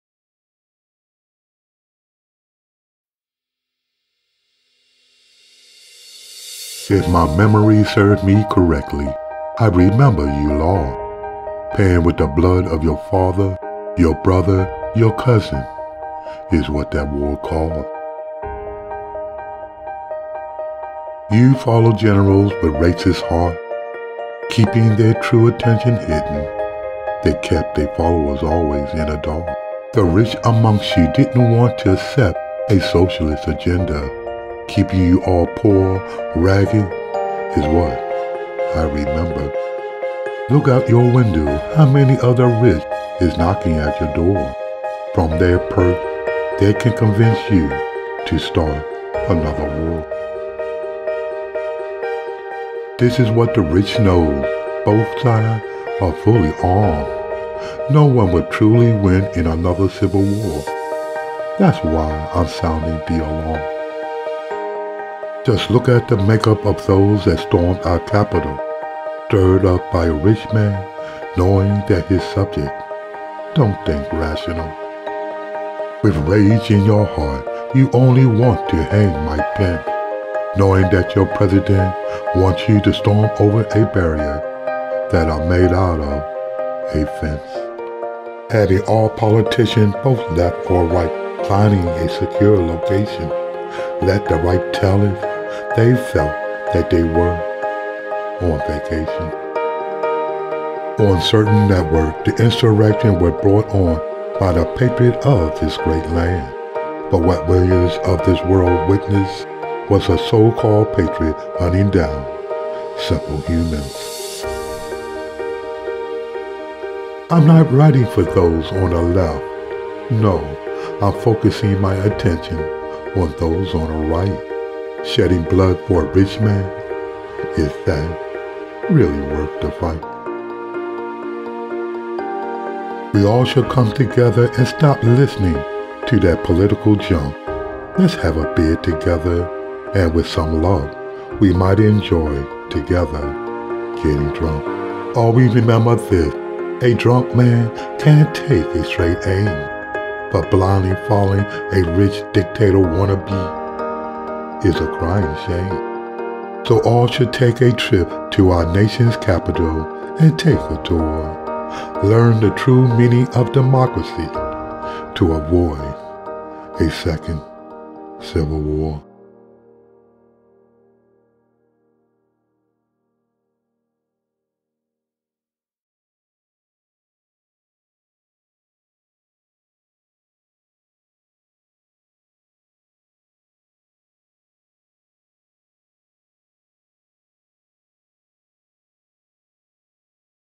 A Second Civil War
A Second Civil War was written to enlighten those that were lead astray. How can only one man capture so many minds. If we don't get control of this, then you can kiss democracy good-by. A second civil war will be a reality if we don't stop this man. Supreme court justices must know that a dictator don't need a court system to place checks to his power. Let see what this November will bring.